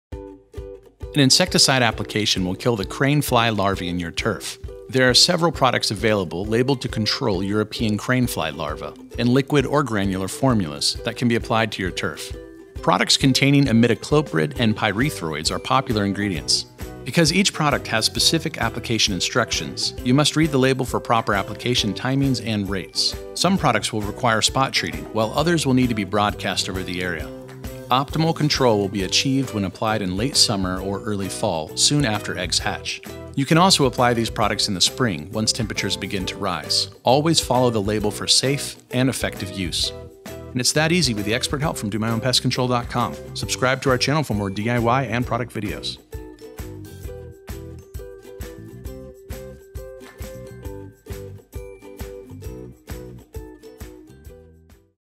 How to Get Rid of Crane Flies
...Click here for Control Products!

An insecticide application will kill the crane fly larvae in your turf. There are several products available labeled to control European crane fly larvae, in liquid or granular formulas, that can be applied to your turf.